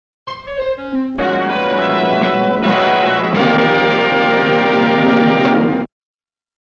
TTAFAN Productions Logo Classic Version (Remake)
This is The Remake of The Classic Logo.
Audio Used:United Artists Television
Picture Used:Rick Astley (The Man that Rickrolls You)